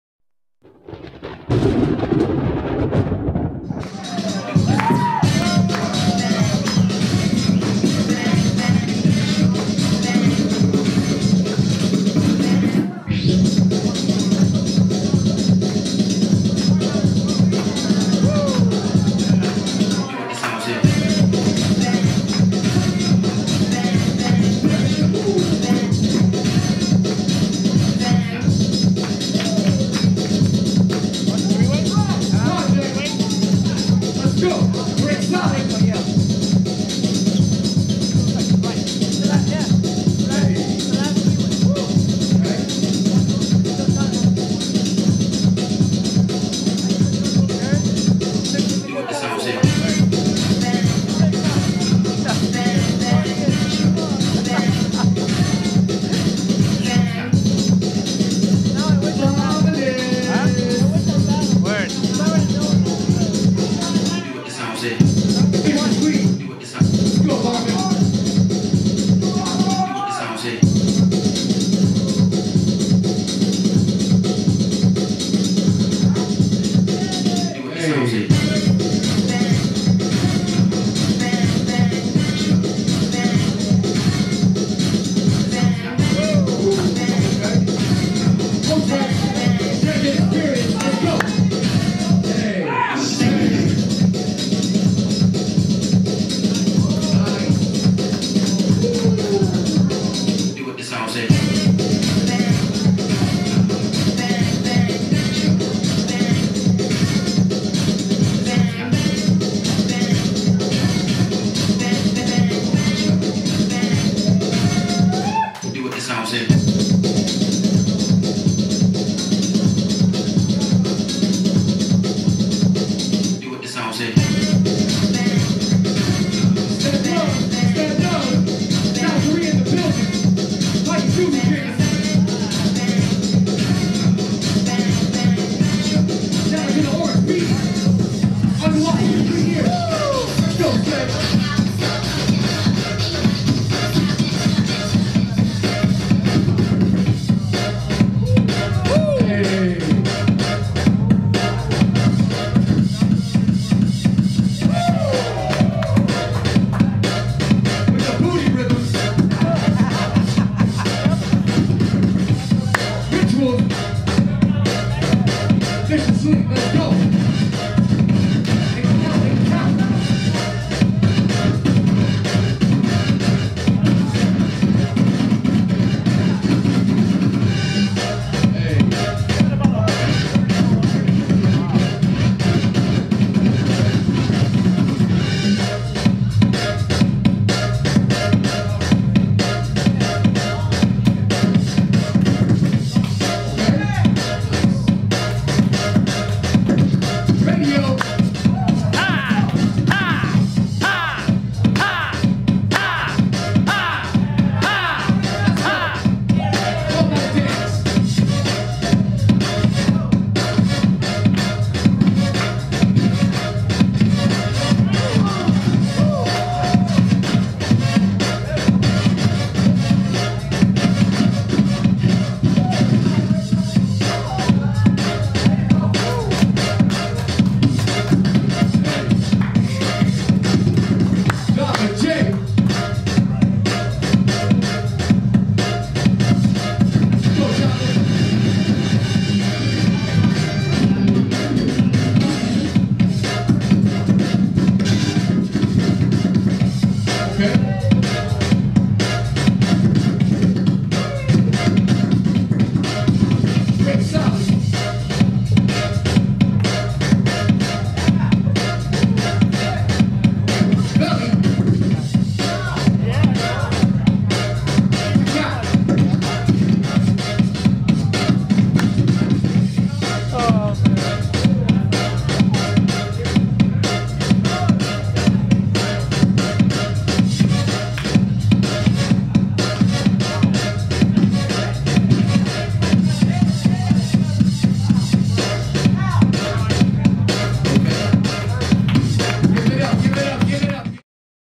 Twisted Kicks Part Deux Prelims
Prelim Battles

Project, Sted Love, Jazzy jay
vs
Marvilous Marv, Rituals, Veli
vs
Jello Jam, Radio, Oteq